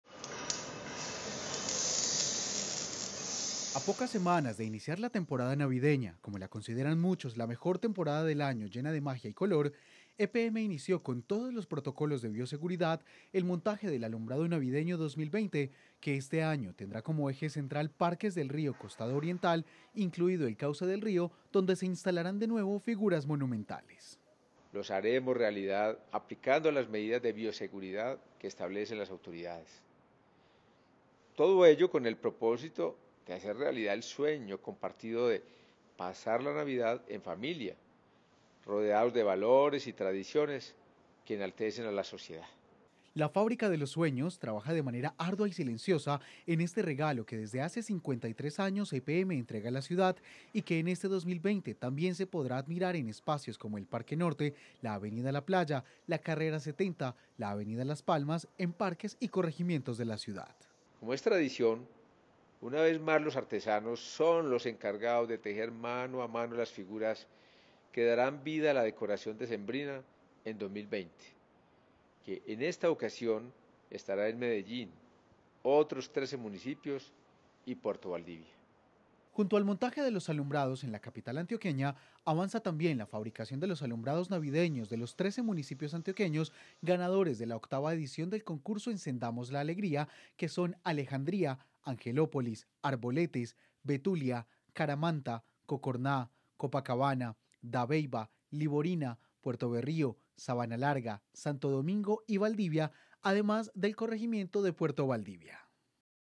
Inició el montaje del alumbrado navideño 2020 - Telemedellín
Con todos los protocolos de bioseguridad, inició el montaje del alumbrado navideño 2020. Más de 100 personas trabajan en diferentes grupos de trabajo en el diseño, construcción y montaje de este emblemático regalo de EPM para la capital antioqueña, además, este año son 13 los municipios ganadores de la octava edición del concurso Encendamos la Alegría.